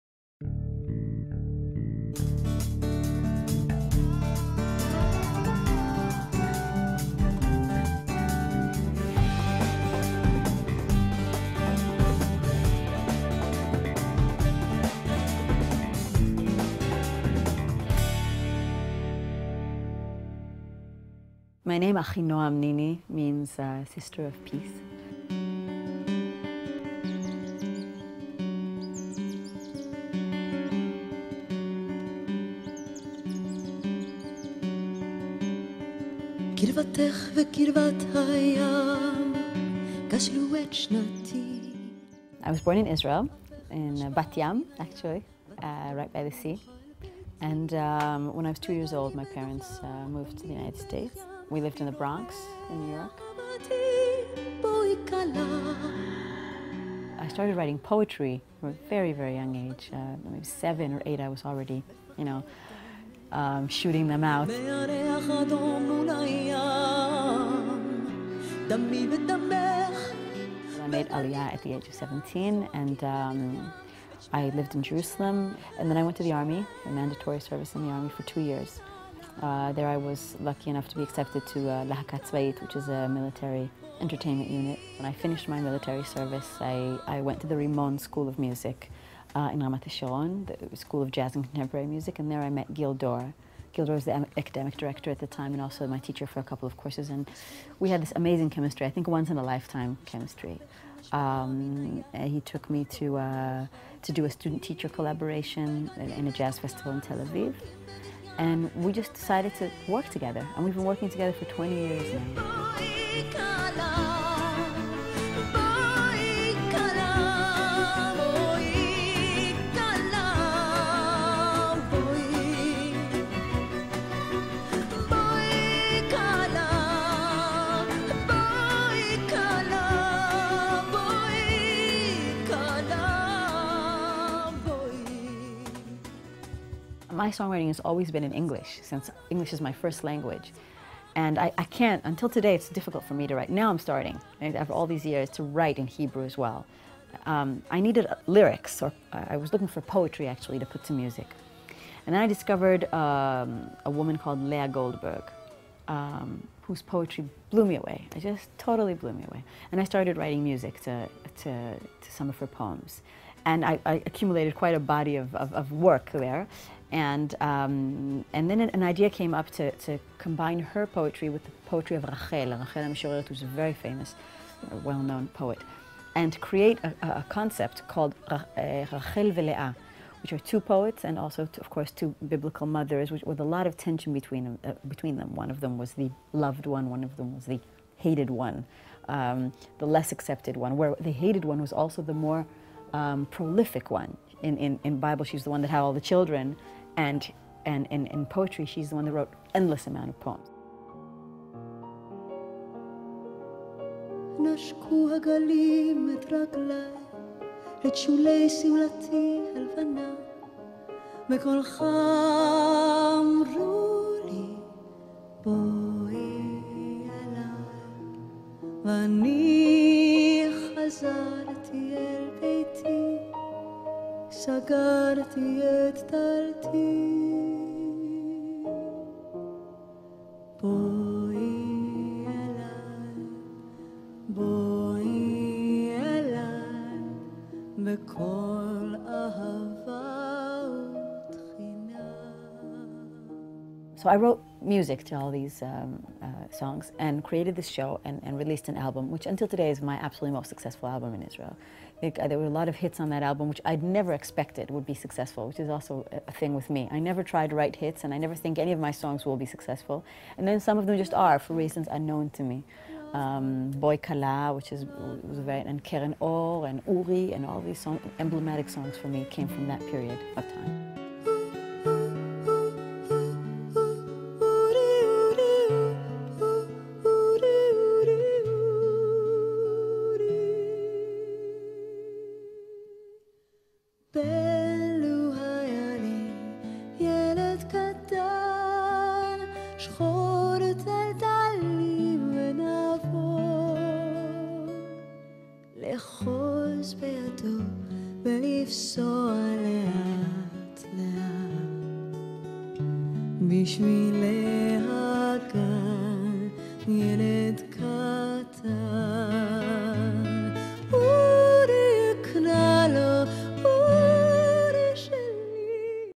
Extract from the Interview with Noa (Achinoam Nini) at Shalom Tv (Muzika)
Israel's leading international concert and recording artist, Noa (known in Israel by her given name of Achinoam Nini) combines her Yemenite roots with 1960s musical influences Paul Simon, Joni Mitchell, and Leonard Cohen. A Shalom TV exclusive series.
A new series exploring the most unique, interesting, and powerful voices and music being produced in Israel today. Coming in early 2011.
Director/Producer/Editor: Igal Hecht
Camera/Co-producer: Lior Cohen
Copyright Chutzpa Productions Inc. 2011